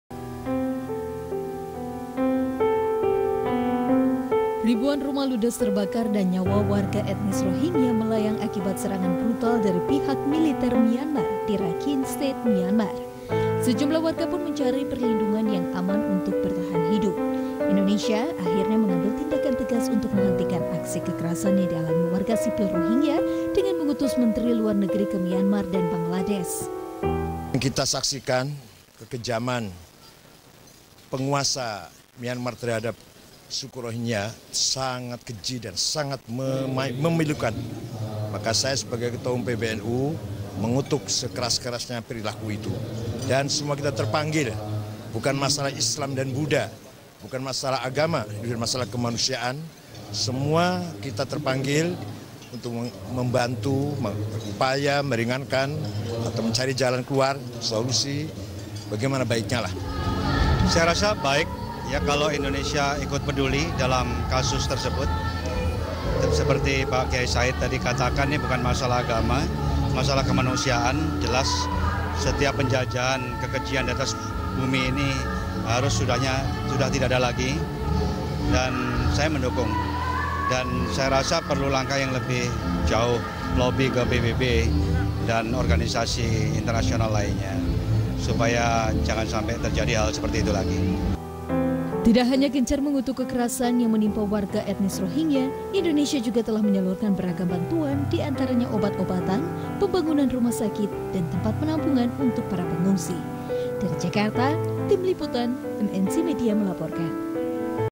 Pembantaian Rohingnya, HT: Kekejian harus tidak Ada lagi
Pemerintah dan masyarakat Indonesia mengecam tindakan militer Myanmar yang membantai etnis Rohingya. Ketua Umum Partai Perindo juga menyampaikan kecaman tersebut dan mendukung upaya pemerintah RI menghentikan kekejian tersebut.